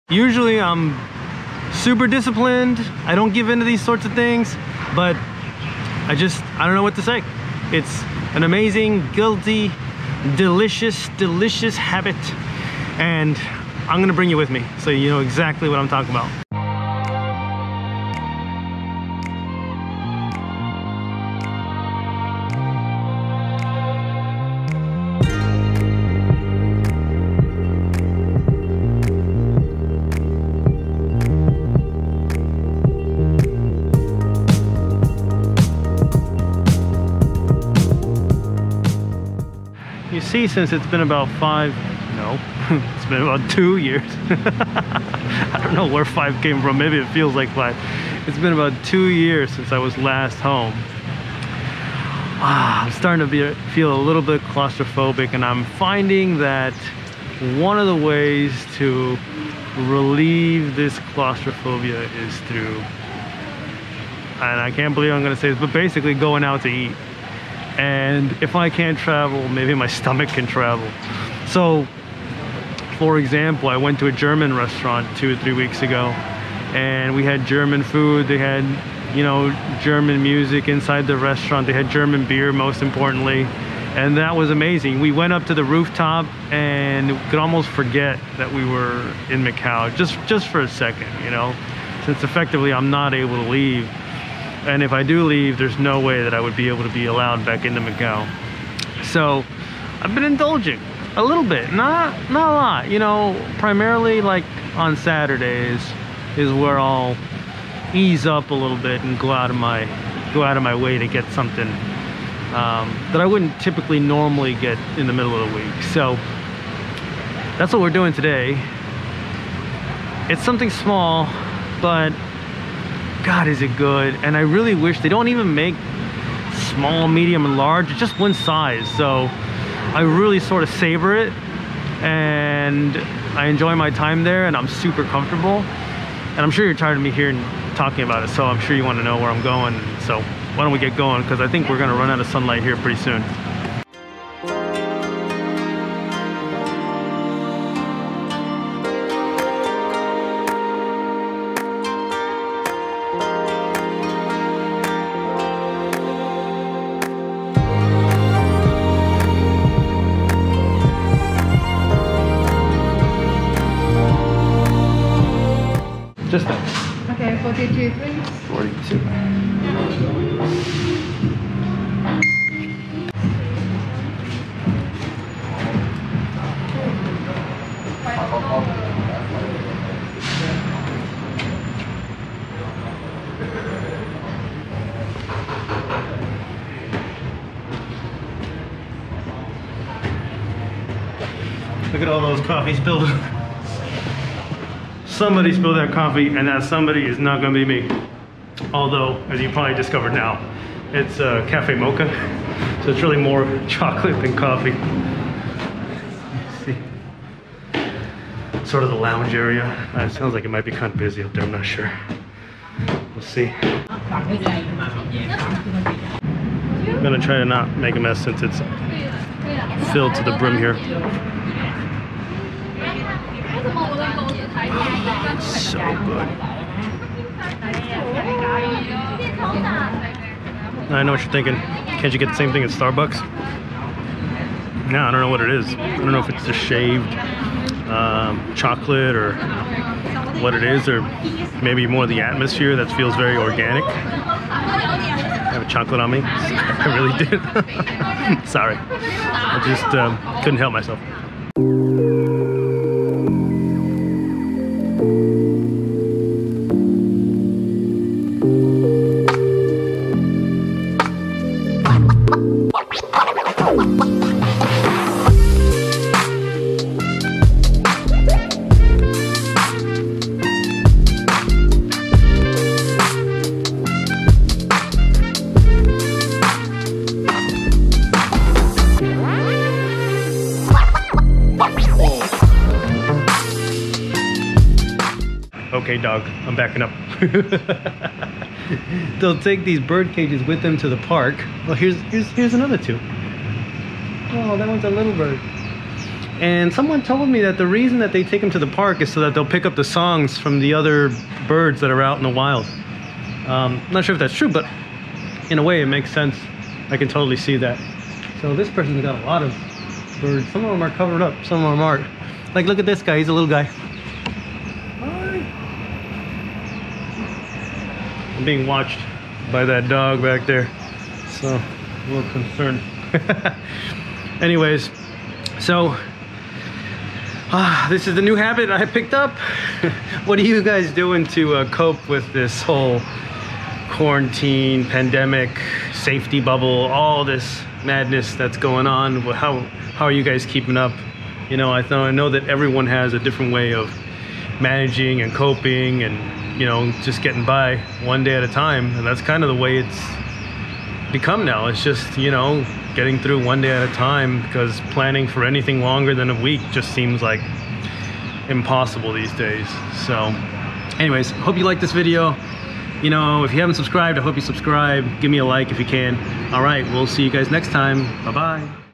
Macau - a local COFFEE house worth VISITING
Tucked away near Senado Square there is a place in the heart of Macau that needs to be visited, a relaxing place to pass the time.